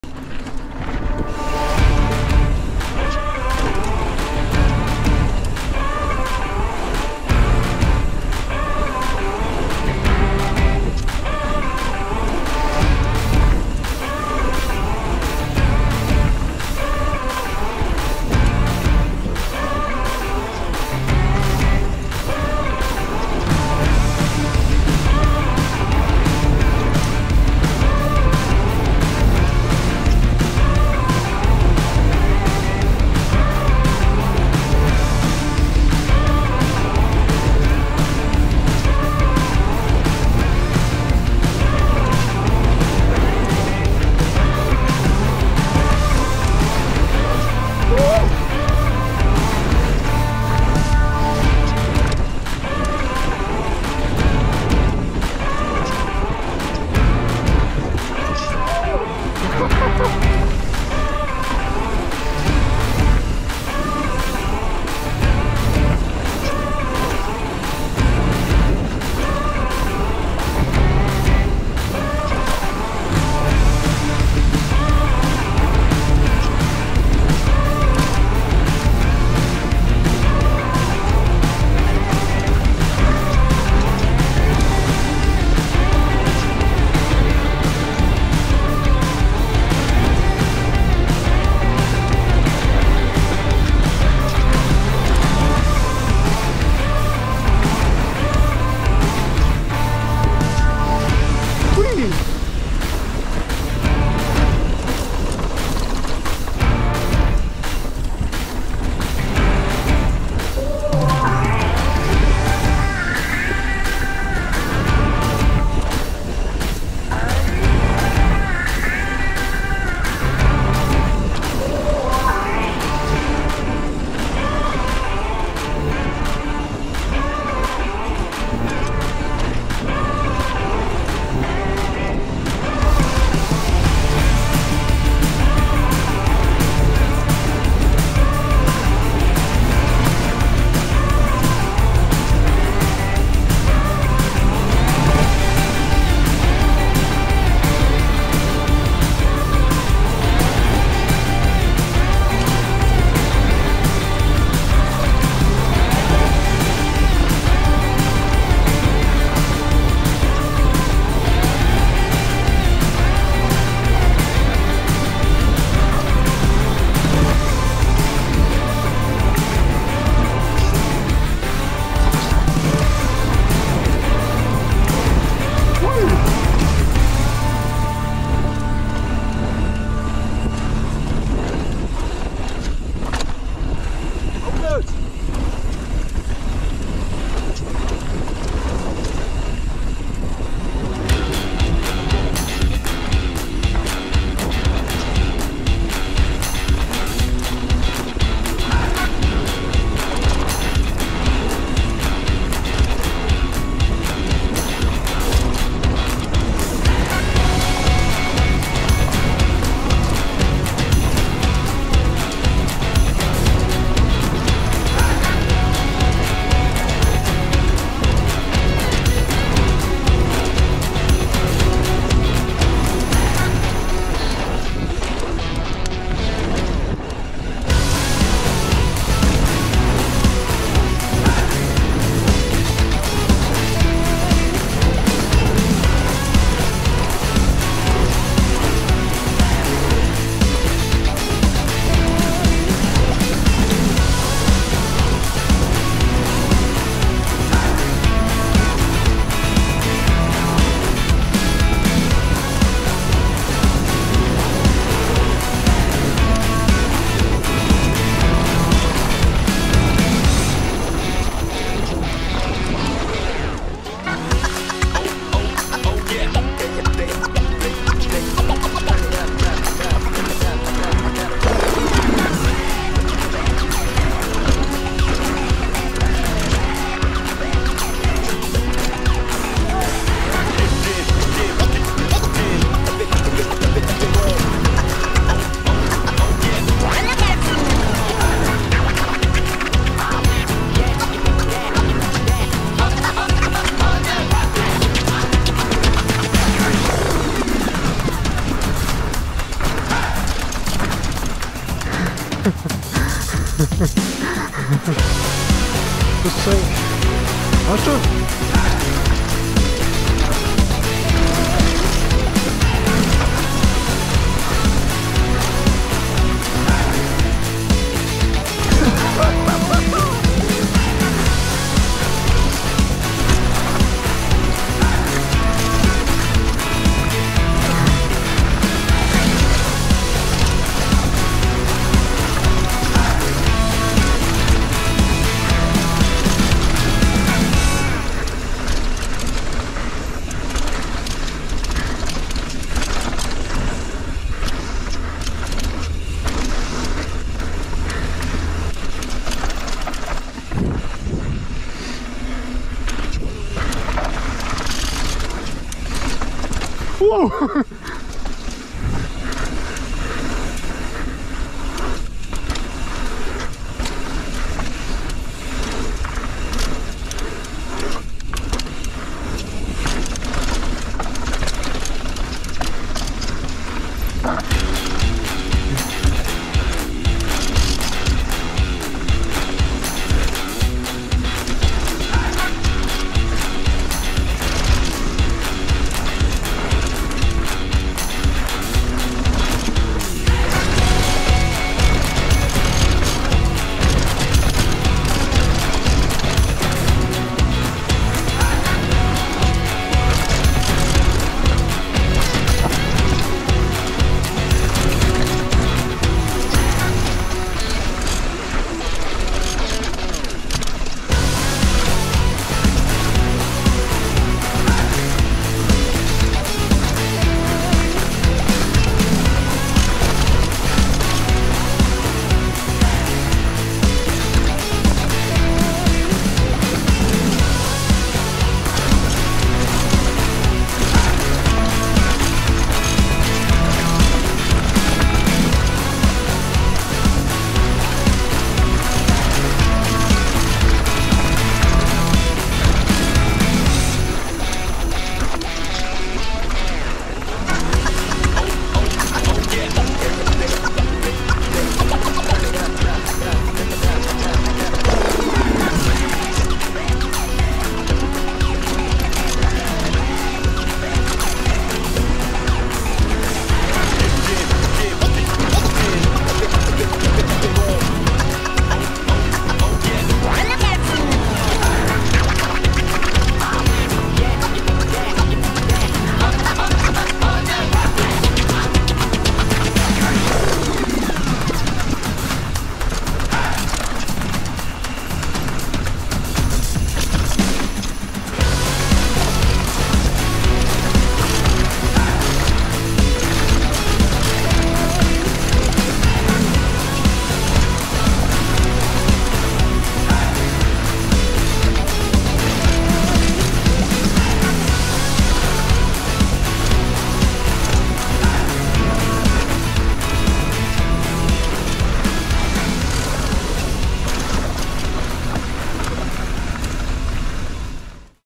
Flowline to Downtown: Epic Fun at Bikepark Schladming
In unserem spannenden YouTube-Video "Flowline to Downtown: Epic Fun at Bikepark Schladming" erleben wir eine coole Abfahrt im Bikepark Schladming. Wir stellen die Frage: Bereitet die Flowline wirklich Freude? Absolut! Die Flowline kann sowohl entspannt als auch flott befahren werden. Auf dieser Strecke erwarten dich nicht nur ein angenehmer Fahrfluss, sondern auch einige kleine, dennoch aufregende Sprünge als Belohnung.

Aber es gibt noch mehr zu entdecken: Die Kesslerline und Downtown Line bieten rasante Abfahrten für Adrenalinjunkies. Wer es gemütlicher mag, kann auch die coole Stadium Flowline wählen, um entspannt ins Tal zu gelangen. Das Video zeigt, dass der Bikepark Schladming für jeden Fahrstil etwas zu bieten hat und definitiv für epischen Fahrspaß sorgt.

► Timestamp:
00:00 Start Flowline
04:22 Flowline ab Mittelstation
05:05 Kessler Line - letzter Teil
05:48 Downtown Line bis ins Tal

► Meine Bikes:
Custom Haibike Xduro Nduro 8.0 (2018)
Custom Propain Spindrift (2021)